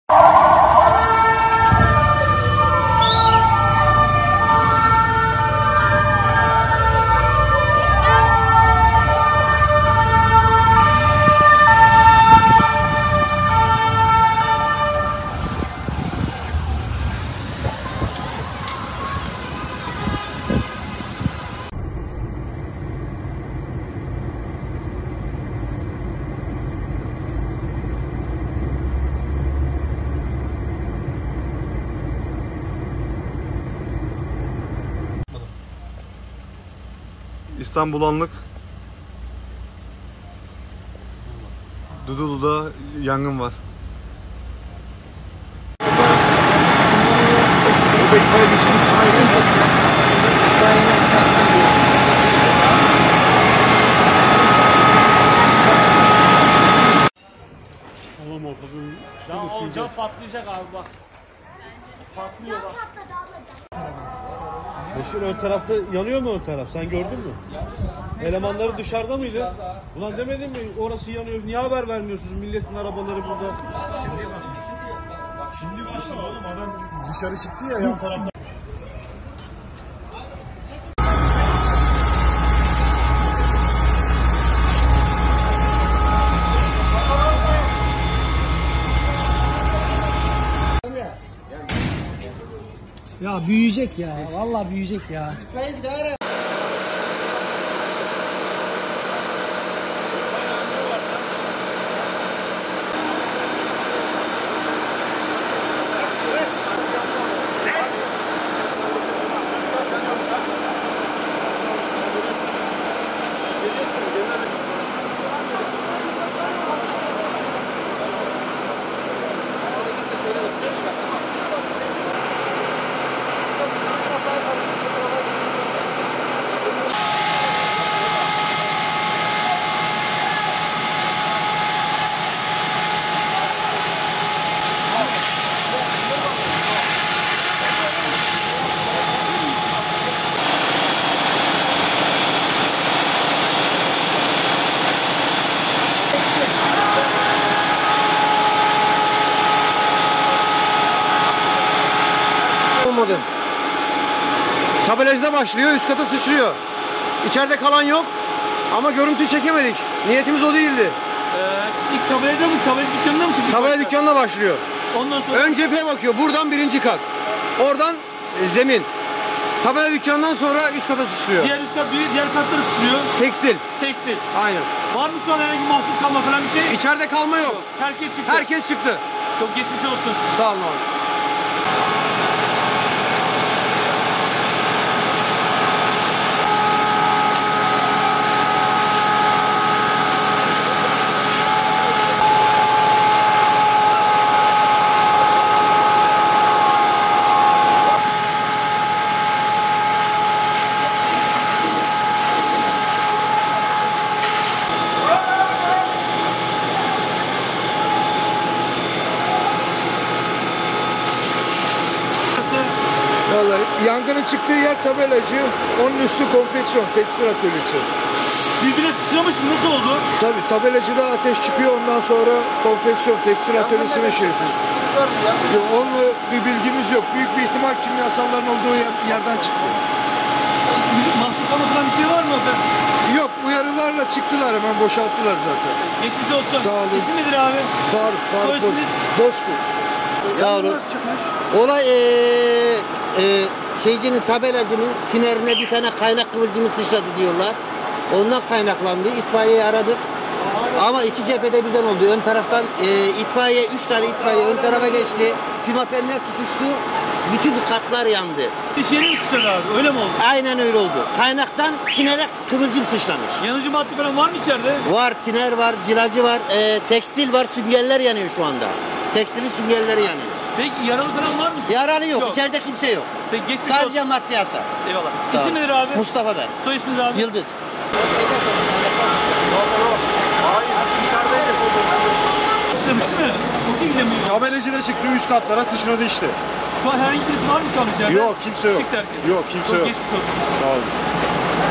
Ümraniye'de İş Yerinde Yangın
ÜMRANİYE’de 4 katlı binanın giriş katında bulunan iş yerinde yangın çıktı. Yangın kısa sürede büyüyerek binadaki diğer işyerlerine sıçradı. İtfaiye ekiplerinin yaklaşık bir saat süren çalışmalarının ardından yangın söndürüldü.
Yangın, 17.00 sıralarında Aşağı Dudullu Mahallesi, Hamza Yerlikaya Bulvarı üzerinde bulunan 4 katlı binada çıktı. İddiaya göre giriş katındaki tabela dükkanında çıkan yangın kısa sürede büyüyerek binadaki diğer işyerine sıçradı. Binada bulunanlar dışarı çıkarken, ihbar üzerine olay yerine çok sayıda itfaiye, sağlık ve polis ekipleri sevk edildi. Olay yerine gelen polis ekipleri güvenlik şeridi çekerek vatandaşları yangın yerinde uzaklaştırdı. İtfaiye ekiplerinin yaklaşık bir saat süren çalışmalarının ardından yangın söndürüldü. Yangında ölen ya da yaralanan olmazken, işyerinde hasar meydana geldi. Alevlerin sardığı ve tabelacı olduğu belirtilen iş yerindeki yangın çevredekiler tarafından cep telefonu ile görüntülendi. Görgü tanığı Mustafa Akça “Tabelacıda başlıyor yangın sonra üst katlara sıçrıyor. Üst katta tekstil üzerine işyeri. İçeride kimse yok, herkes dışarı çıktı” diye konuştu. Yangınla ilgili soruşturma başlatılırken, yangının çıkış nedeni yapılacak çalışmaların ardından belirlenecek.